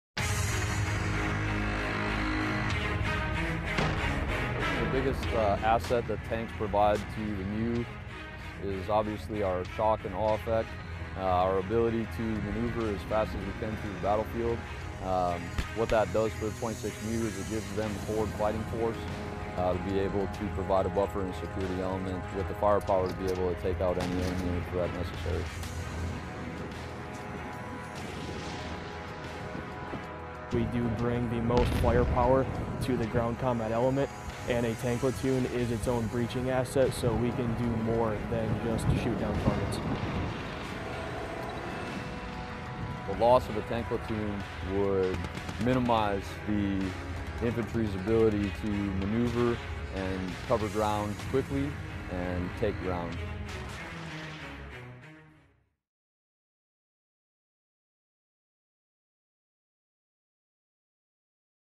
Tank Takeover | 26th MEU tank operations
Marines with the 26th Marine Expeditionary Unit conduct tank operations in the 5th and 6th Fleet area of operations, April 2-June 20, 2018.

(U.S. Marine Corps video by Sgt. Sylvia L. Tapia)